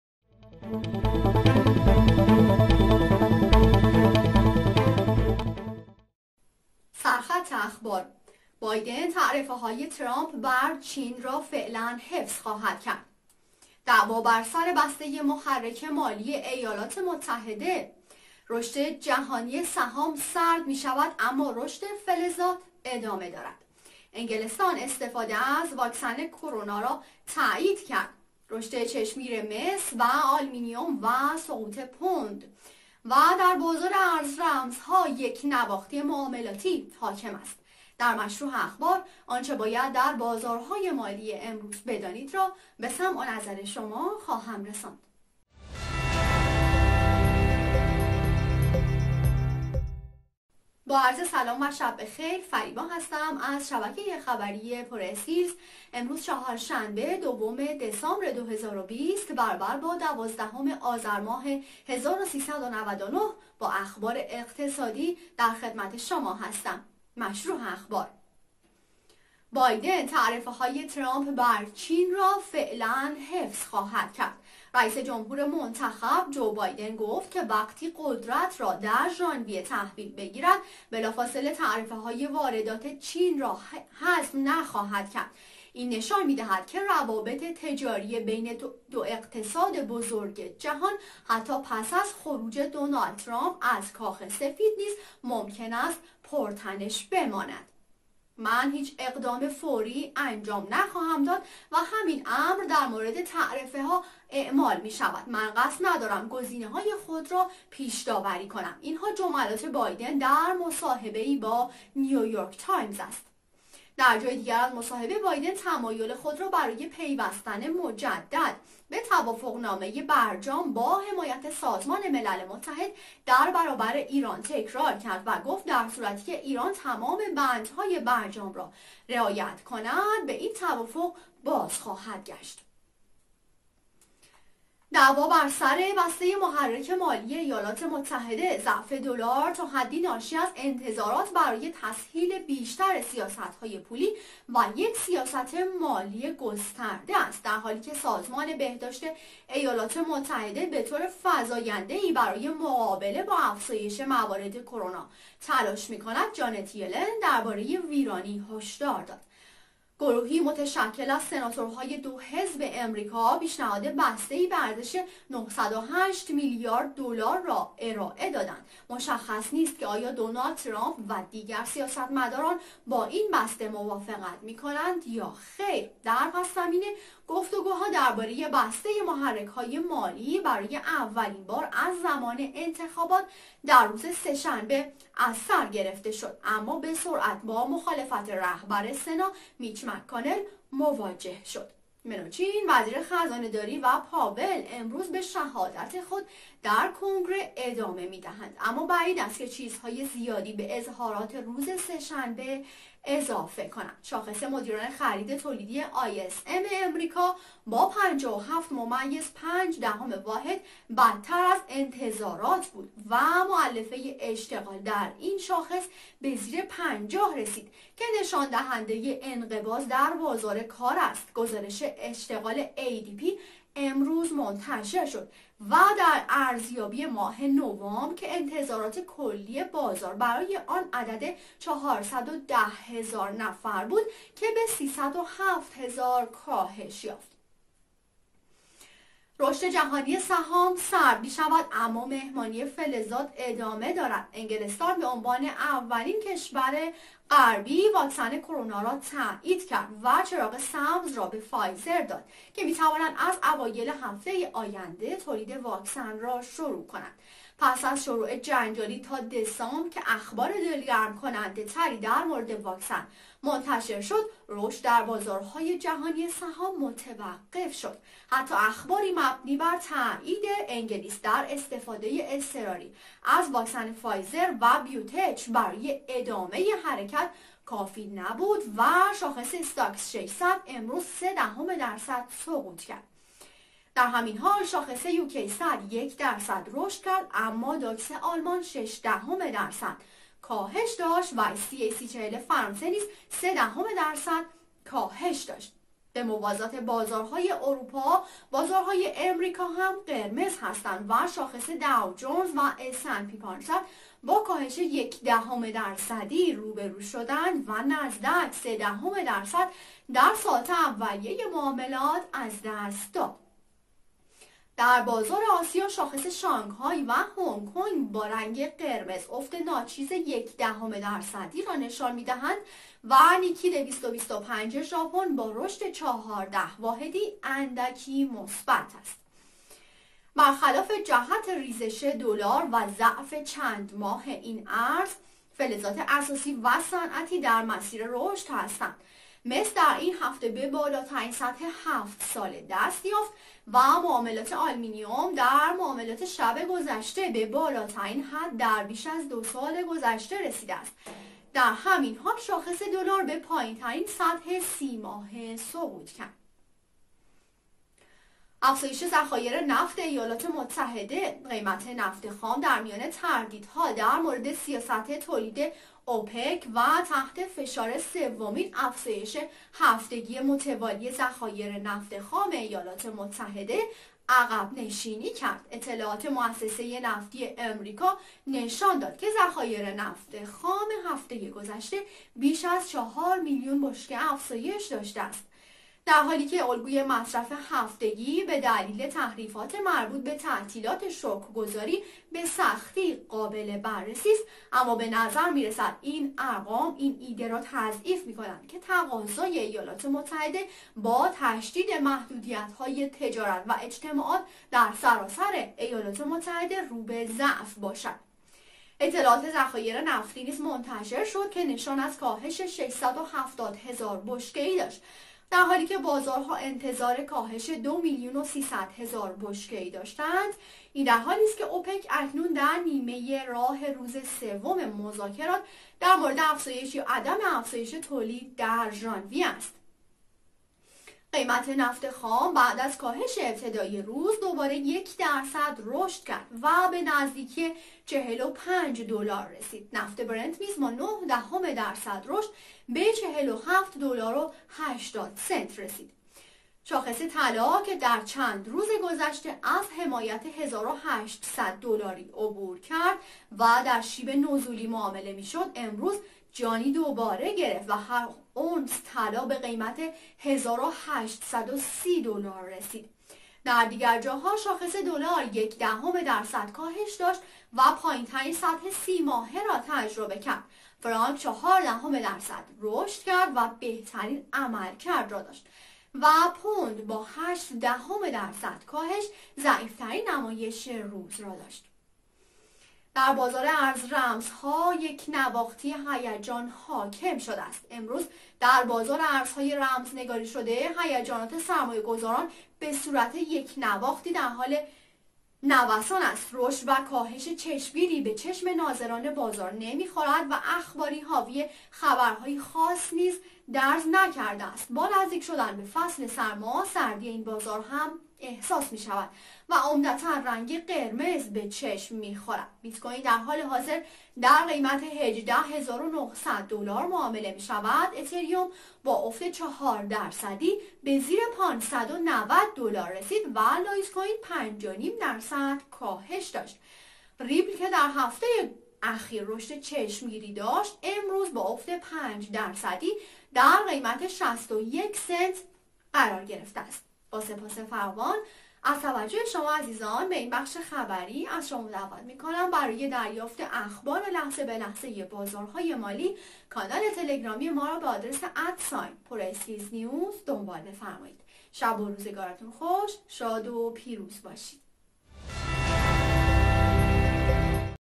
اخبار_اقتصادی - ۲ دسامبر ۲۰۲۰
بازار های سهام سرد شدند!

بایدن تعرفه های ترامپ بر چین را فعلاً حفظ خواهد کرد. دعوا بر سر بسته محرک مالی ایالات متحده. رشد جهانی سهام سرد می شود، اما رشد فلزات ادامه دارد. انگلستان استفاده از واکسن کرونا را تایید کرد. رشد چشمگیر مس و آلومینیوم  و سقوط پوند و در بازار ارز رمزها یکنواختی معاملاتی حاکم است.